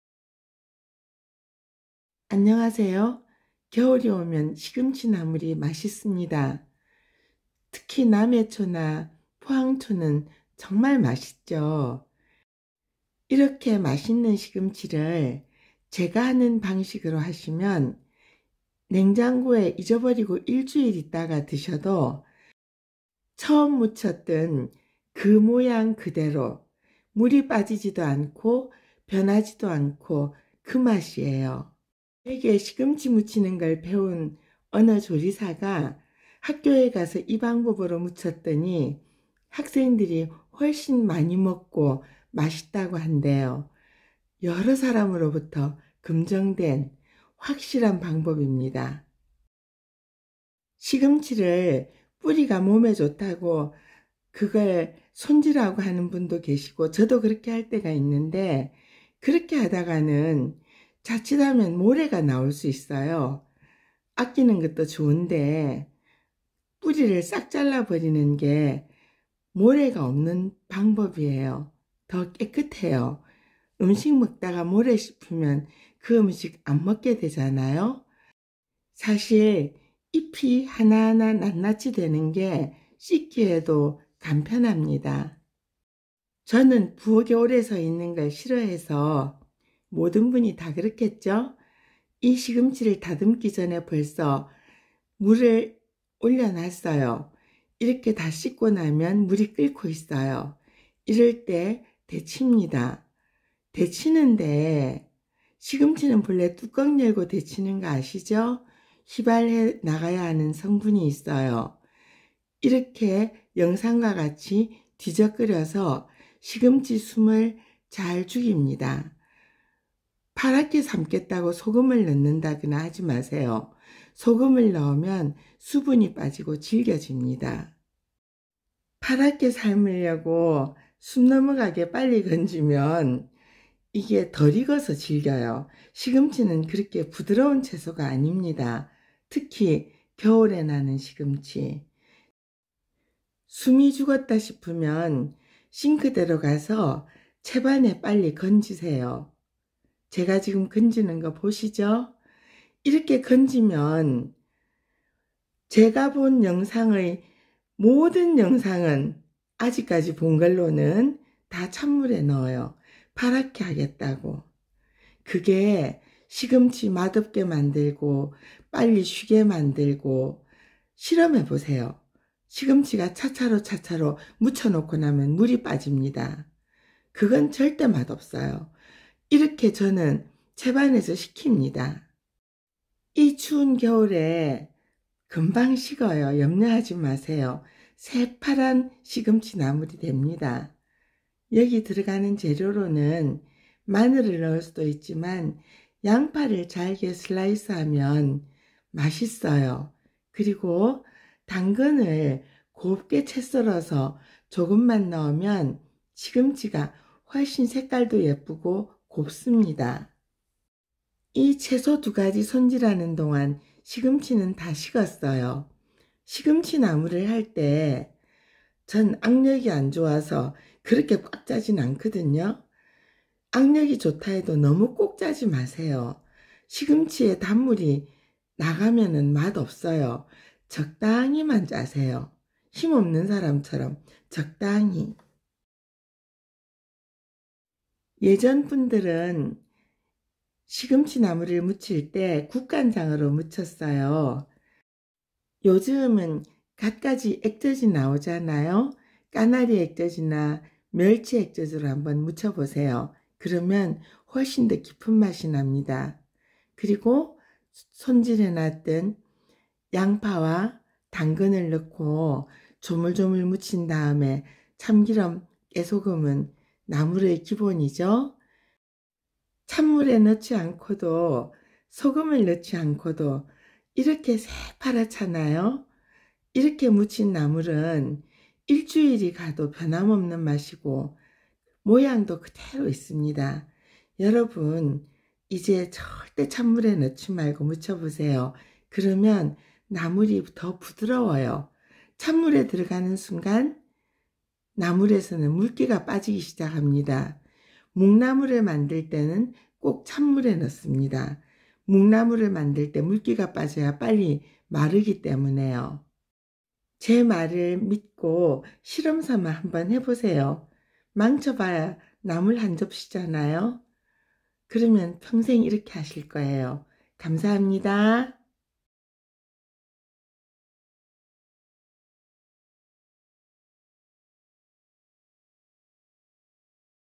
제발 시금치 나물은 이렇게 해 보세요. 맛있어요. 잘 안 쉬어요. 부드러워요
시금치는 몸에 좋은 대표채소지만 수산이 있어 반드시 데쳐야 합니다.
수산은 결석을 만듭니다.
우유는 몹시 상극 음식입니다.
파랗게 한다고 30초 데치라는 유튜버도 있지만 수산 제거가 안되어서 3~4분 데쳐도 됩니다.
생각만큼 누렇게 되지 않아요.

시금치나물 잘 쉬지 않는 시금치 무침 부드러운 시금치 무침